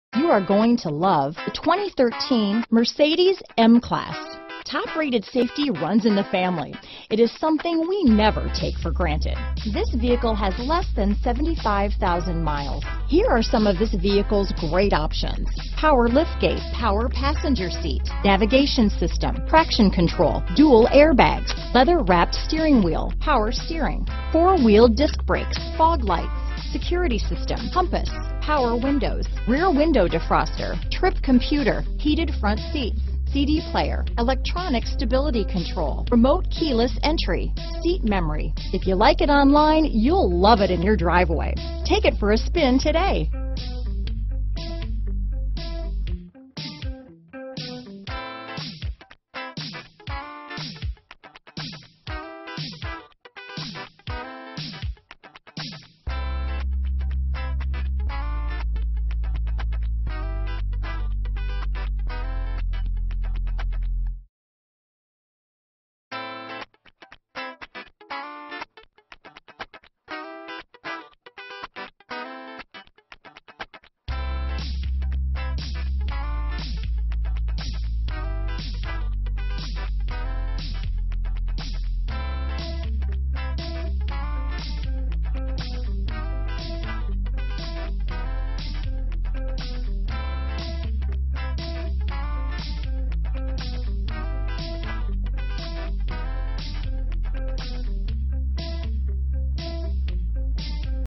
2013 Mercedes-Benz M-Class Hampton, Chesapeake, Williamsburg, Yorktown, Newport News, VA 417361P
Palladium Silver Metallic U 2013 Mercedes-Benz M-Class available in Newport News, Virginia at Suttle Motor Corporation. Servicing the Hampton, Chesapeake, Williamsburg, Yorktown, VA area.